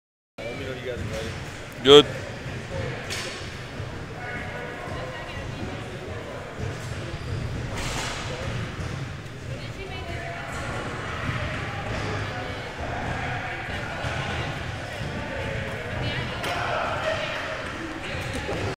Dartfish Project video 2 Decline with Weights
Project Dartfish.com comparison of regular bench press to decline bench press for power, force, acceleration, angular inertia/ momentum, and muscle involvement at Towson University Kinesiology Department